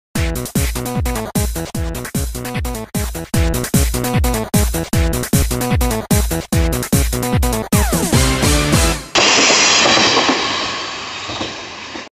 REAPERの練習で作った10秒音源で吹っ切れた
セリフ合わせが自分の思う感じにならないのでとりあえず10秒尺作ってみた
イヤホン付けることオススメ